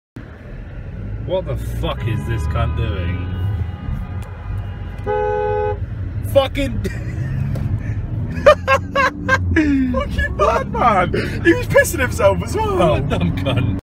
Old Man Cruises on Scooter, Lives the Dream || ViralHog
Info from Licensor: "My friend and I were just cruising down the dual carriageway, when I just heard him  say "What the F! Turn around now!" I turned around, got a glimpse of the old dude on the scooter and instantly got my phone out to record it. You've seen the rest." - Conor Allen-Miles

Occurred: May 14, 2016 / Oxfordshire, UK